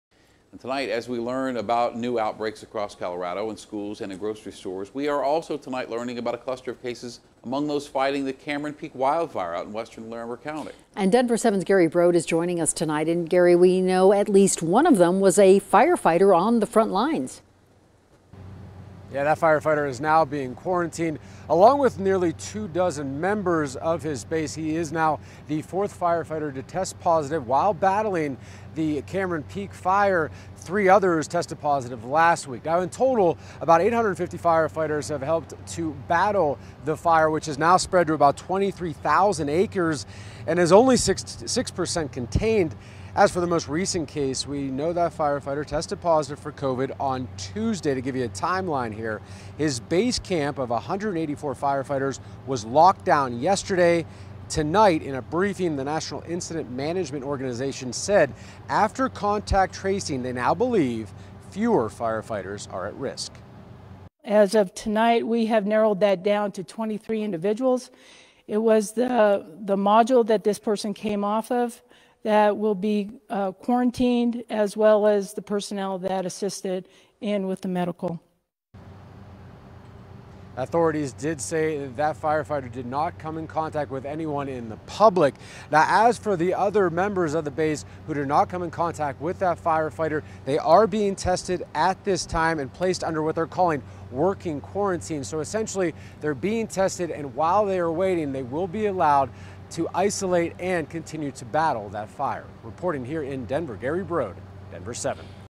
Firefighter battling Cameron Peak Fire tests positive for COVID-19, dozens potentially exposed
A forward operating base for the Cameron Peak Fire was shut down after a firefighter battling the blaze tested positive for COVID-19 earlier this week, potentially exposing more than 30 other people at the base, according to officials with the Rocky Mountain Incident Management Team.